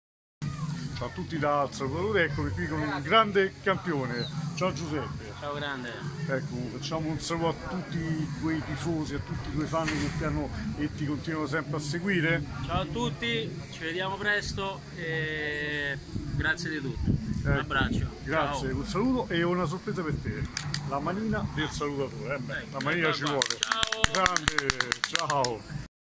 Il Campione del mondo di salto con l’asta Giuseppe Gibilisco saluta i fan col Salutatore
«Colpo di testa all’Alzheimer» 29 giugno 2019  a Cerveteri, allo stadio comunale Enrico Galli, alle ore 15.00,un evento calcistico di beneficenza per sostenere la «Fondazione Sanità e Ricerca» di Roma nell’assistenza  alle persone con Alzheimer.
la nazionale italiana «All Star» in campo contro la «ASD Nuovo Borgo San Martino» 3-3 ai rigori vincono le All Star : Il campione del mondo di salto con l’asta Giuseppe Gibilisco saluta i fan col Salutatore

➜ FACEBOOK IL SALUTATORE:
➜ PAGINA